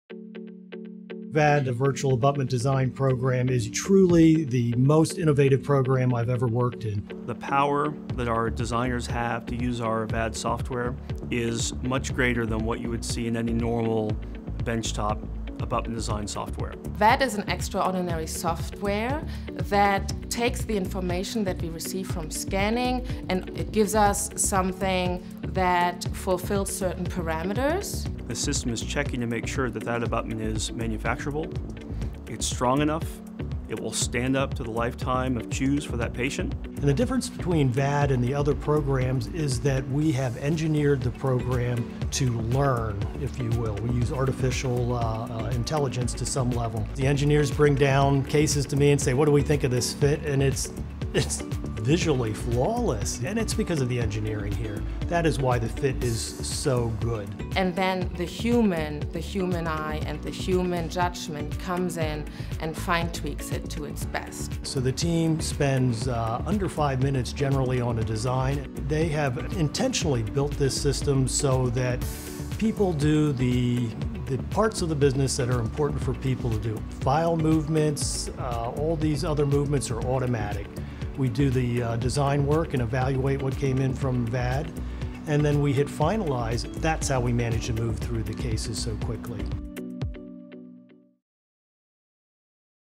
Virtual Atlantis Design (VAD) - Artificial Intelligence
The use of artificial intelligence in implant dentistry is no longer a theoretical concept, it's happening right now with Virtual Atlantis Design (VAD).